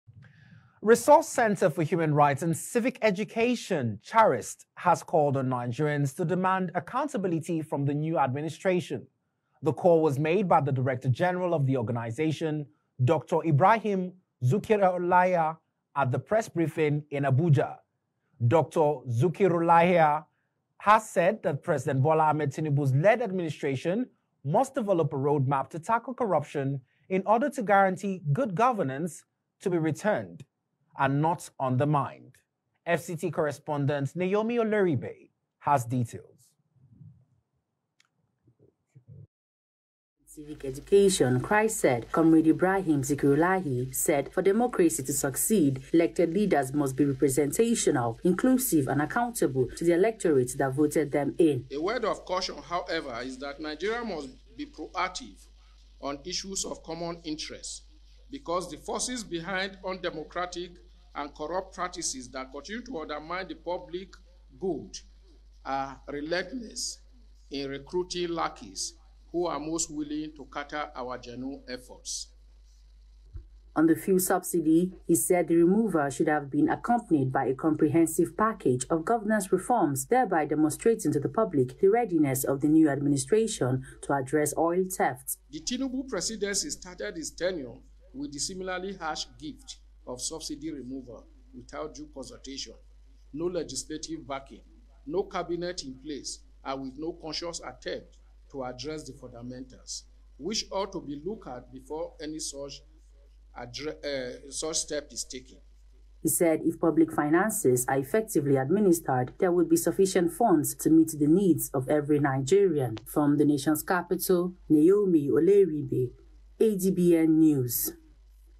STATE OF THE NATION - Citizens Must Demand Accountability From New Administration - CHRICED.
RESOURCE CENTRE FOR HUMAN RIGHTS AND CIVIC EDUCATION
(CHRICED) HAS CALLED ON NIGERIANS TO DEMAND
ACCOUNTABILITY FROM THE NEW ADMINISTRATION.

THE CALL WAS MADE BY THE DIRECTOR GENERAL OF THE
ORGANISATION DR IBRAHIM ZIKIRULLAHI AT A PRESS BRIEFING IN
ABUJA.

DR ZIKIRULLAHI SAID THE PRESIDENT BOLA TINUBU LED
ADMINISTRATION MUST DEVELOP A ROAD MAP TO TACKLE
CORRUPTION IN ORDER TO GUARANTEE THAT GOOD GOVERNANCE
WILL NOT BE UNDERMINED.